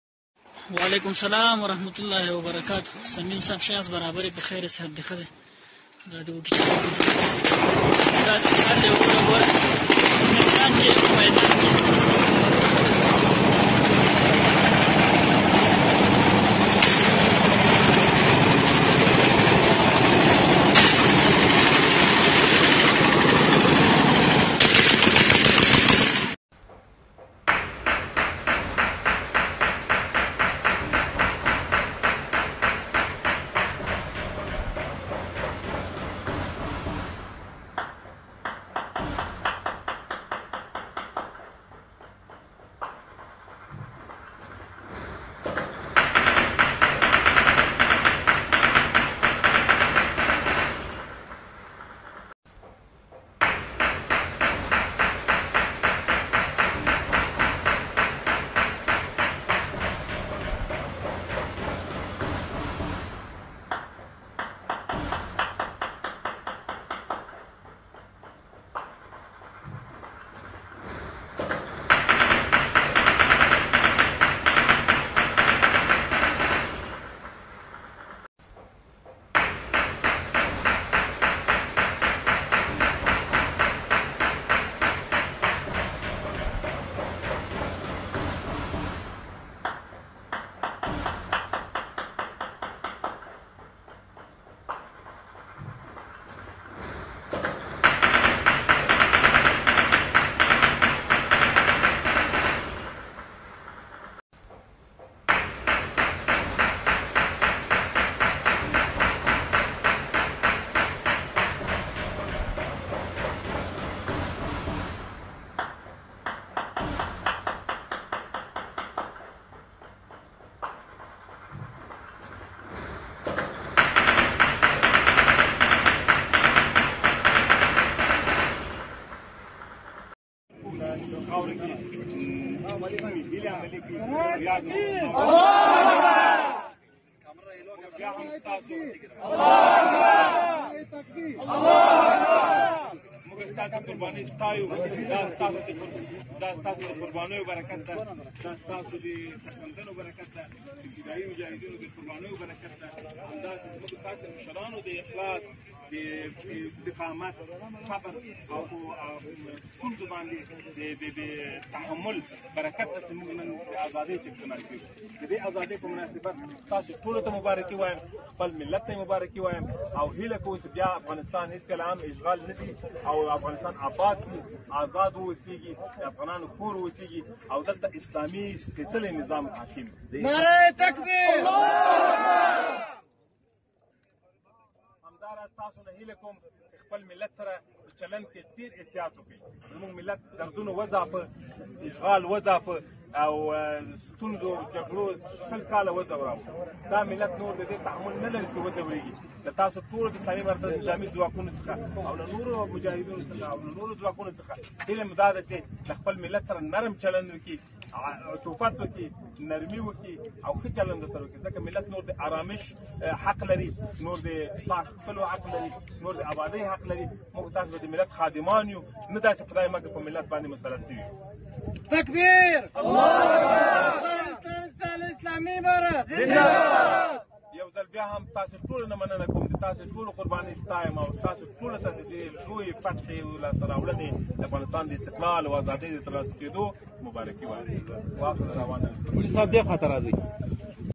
After leaving US Troops Celebrations By Talibanes At Kabul Airport | Fired Into The Air
Taliban's Take Control over Kabul Airport | Last Flight Take Off Yesterday Mid Night By US Airforce After That Taliban's Take Control Over Kabul Airport

Listen With Headphones TroopsLeaves

Now House-to-house executions in Kabul following US mil departure : From senior US source There are no words for what this administration has done to all of Us : Afghan and American